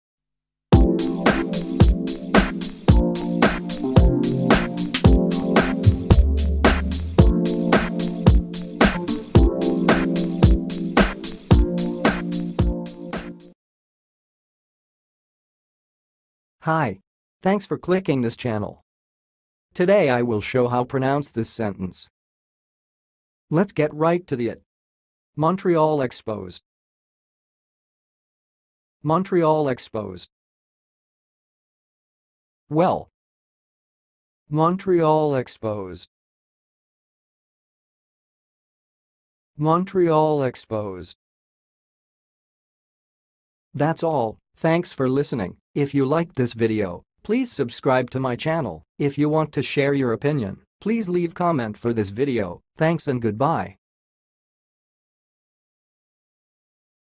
MONTREAL EXPOS - HOW TO PRONOUNCE IT!?
Watch how to say and pronounce "montreal expos"!